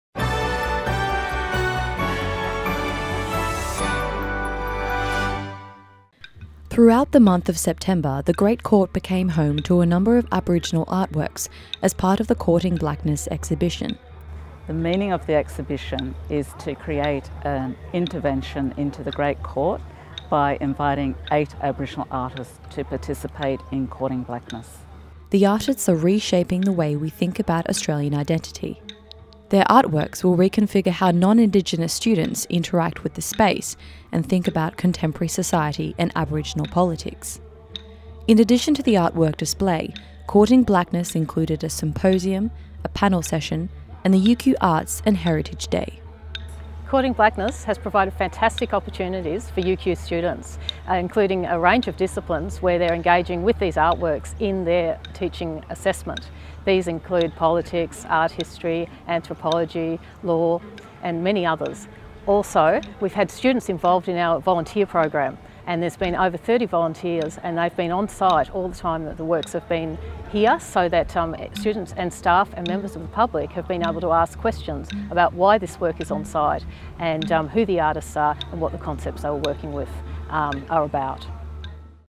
Courting Blakness
The latest episode of UQ News TV features Courting Blakness: Recalibrating Knowledge in the Sandstone University. During the month of September The University of Queensland’s Great Court was home to Courting Blakness, an art installation curated by UQ Adjunct Professor Fiona Foley, that reinterpreted the space through the work of eight Aboriginal artists.

Artists: Michael Cook, Christian Thompson, Karla Dickens, Megan Cope, Natalie Harkin, r e a, Ryan Presley and Archie Moore